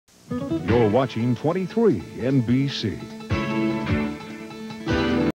KNDO Station ID 1998
This is KNDO's station ID from 1998
KNDO is located in Yakima, WA